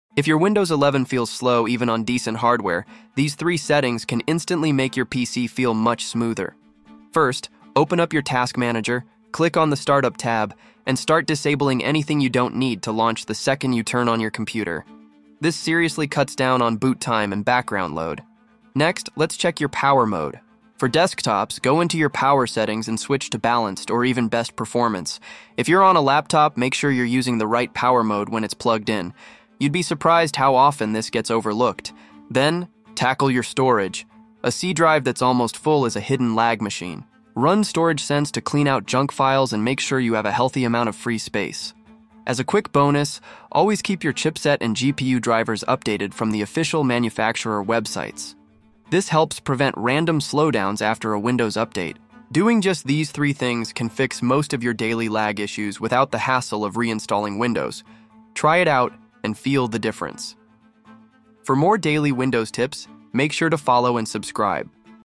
Windows 11 Lag Fix 2025: 3 Settings That Instantly Feel Faster ⚙️🚀
⚙️🚀 Windows 11 lag in 2025 is frustrating because it often feels random — your PC looks “fine” on paper, but everyday tasks still feel sluggish. Apps open slowly, multitasking stutters, and small delays stack up until your system feels older than it really is.

The good news? You don’t need a reinstall for most cases. You just need a clean, practical checklist that targets real-world performance — not theory.

In this short, we cover **3 fast settings** that can instantly make Windows 11 feel smoother and more responsive:

✅ 1) Startup App Cleanup
Many PCs feel slow because too many apps launch in the background. RGB suites, launchers, cloud tools, and “boosters” can silently steal CPU, RAM, and disk resources. A lean startup often delivers the fastest improvement.

✅ 2) Power Mode That Matches Your Use
A wrong power profile can limit performance without you noticing. For desktops, Balanced or Best Performance is usually the best choice. For laptops, you’ll get smoother results when you use the right mode while plugged in.

✅ 3) Storage Sense + Healthy Free Space
A packed C drive is one of the biggest hidden causes of Windows 11 lag. Windows needs breathing room for updates, caching, and background tasks. Cleaning junk files and keeping healthy free space can remove micro-stutters and speed up daily responsiveness.

⚡ Bonus Tip
We also highlight why **chipset + GPU driver hygiene** matters in 2025. Many stability and performance issues come from outdated or poorly installed drivers.

🎯 Perfect for
💻 Laptops and desktops with daily slowdowns
🎮 Gaming PCs that stutter outside games
🛠️ Anyone who wants fast improvements without reinstalling

👉 Try these three steps first — they solve most real-world lag cases.